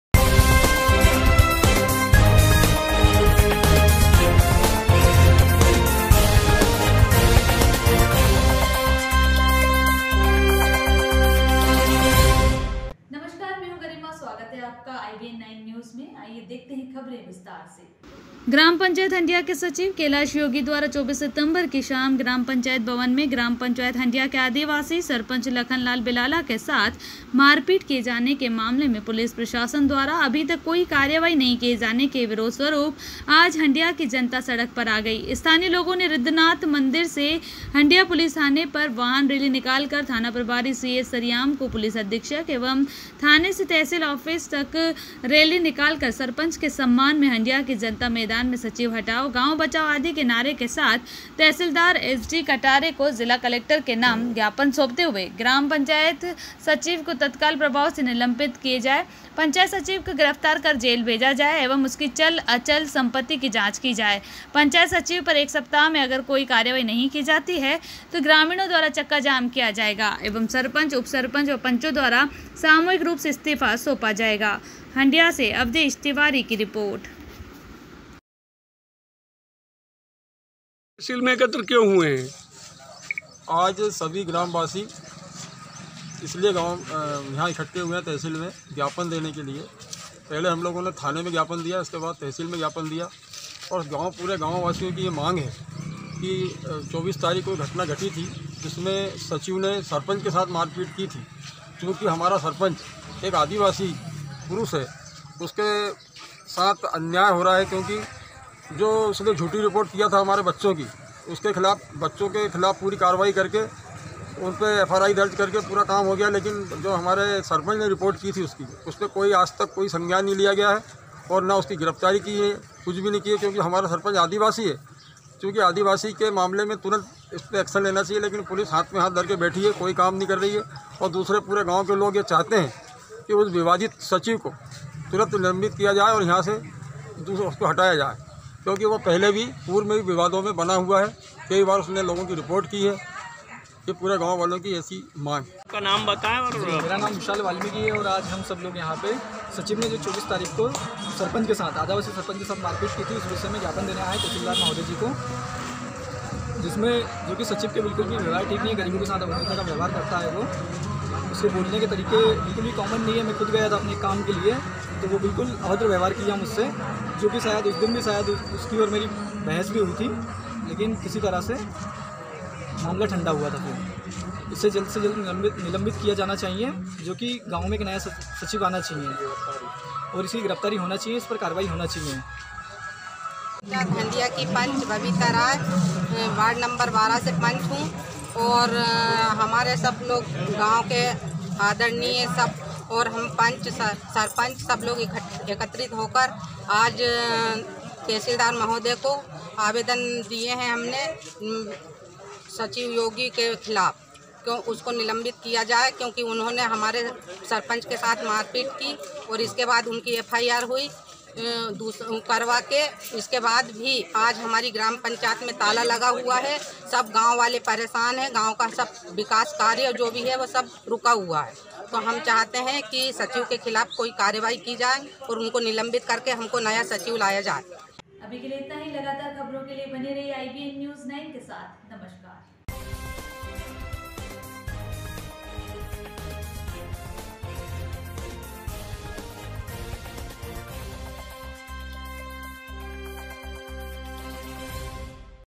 सरपंच के समर्थन में ग्रामीणों ने किया तहसील व थाने का घेराव सौंपा ज्ञापन | IBN9
सरपंच के समर्थन में ग्रामीणों ने किया तहसील व थाने का घेराव सौंपा ज्ञापन